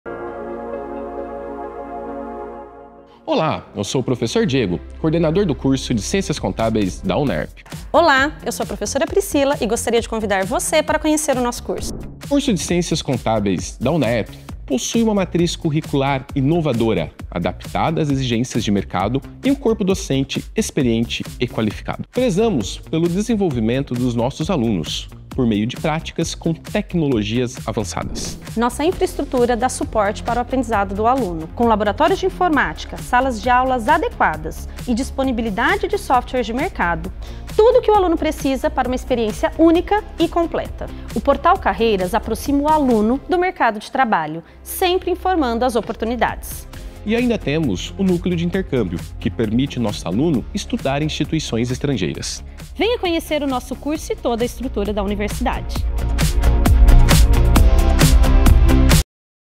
Ciências Contábeis - UNAERP
O Coordenador do Curso de Ciências Contábeis da UNAERP, Diego Bevilacqua Meli , e a profa. Priscila Bertholo, falam sobre o Curso.

Assista e conheça mais sobre o curso de Ciências Contábeis!